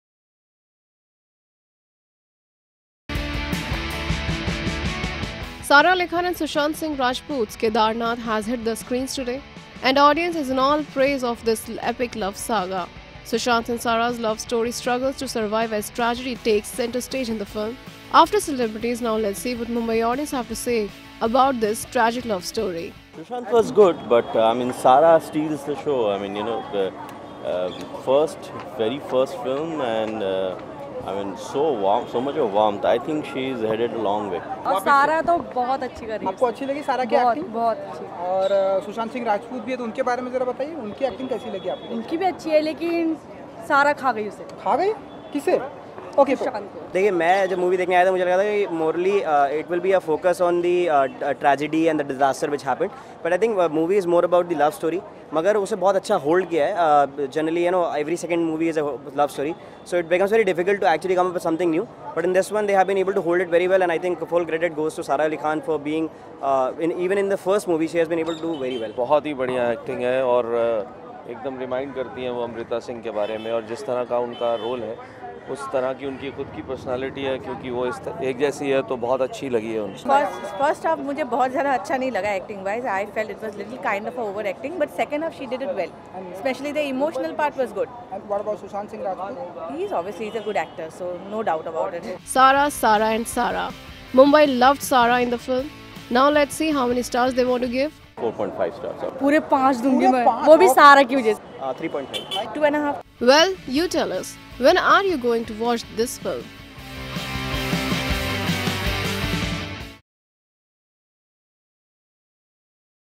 Public REVIEW KEDARNATH | Sara ali khan Steals the show | Sushant Singh Rajput
Sara Ali Khan and Sushant Singh Rajput's Kedarnath has hit the screens today and audience is in all praise of this epic love saga.   Sushant and Sara's love story struggles to survive as tragedy takes centre stage in the film.